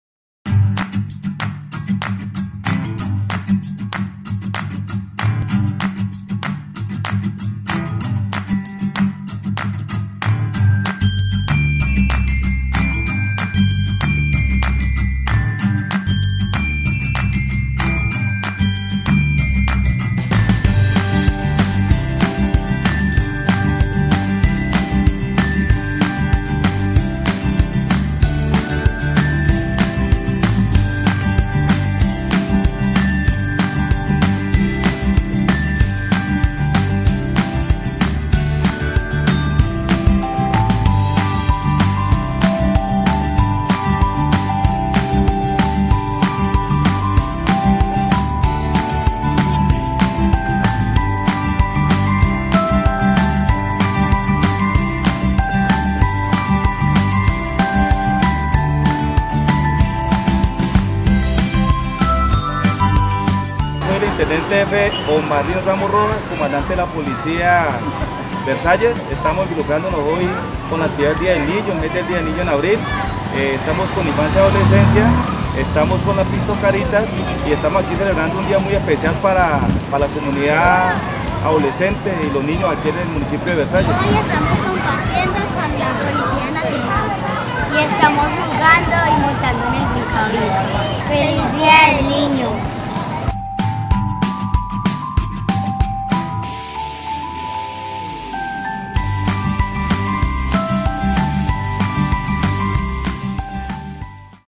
Actividad con la policía nacional
Corporación Juego y Niñez Celebración mes de la niñez en Versalles Valle del Cauca.
Con la colaboración de la Policía Nacional, Alcaldía Municipal e Instituciones del municipio.